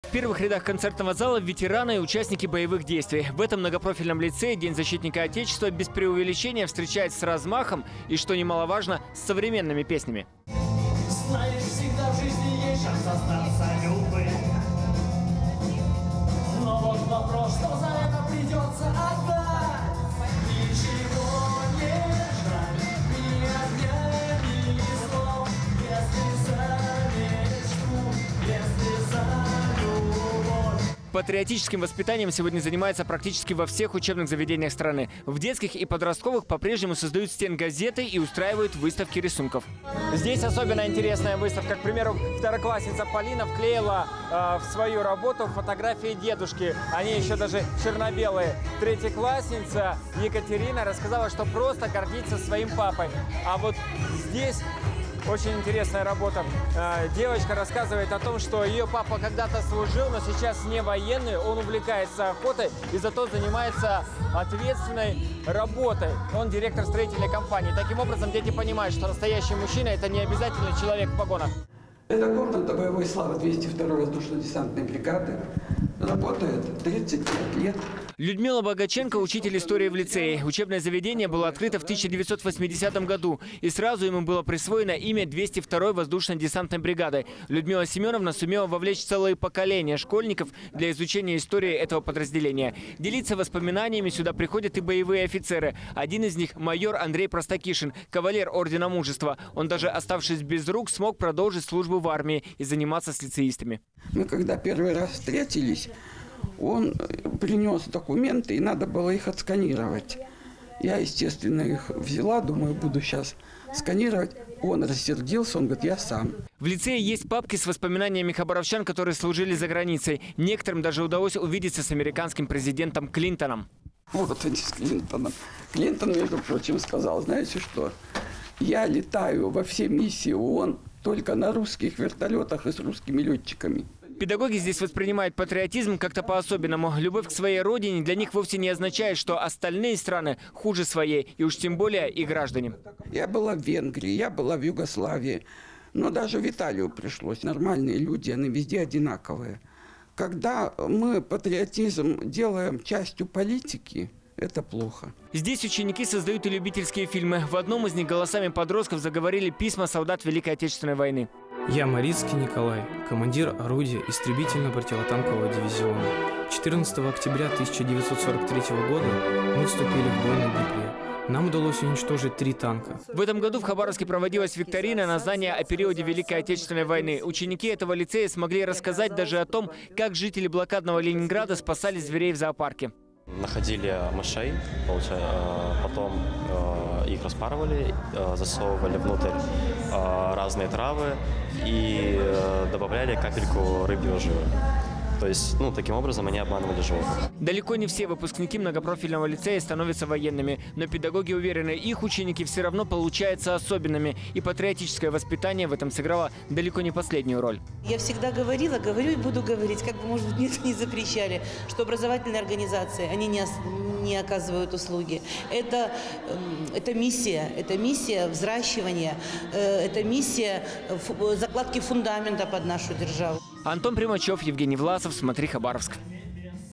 В Хабаровске начались мероприятия ко Дню защитника отечества
Считанные дни остались до празднования «Дня защитника отечества». Социологические центры сейчас традиционно опрашивают граждан о вариантах самого популярного подарка. А в культурных и образовательных учреждениях уже сегодня проводятся торжественные мероприятия. И всё же не везде праздник готовится по обычному сценарию. В многопрофильном лицее «Имени 202 воздушно-десантной бригады» к патриотическому воспитанию учащихся подошли по-особенному.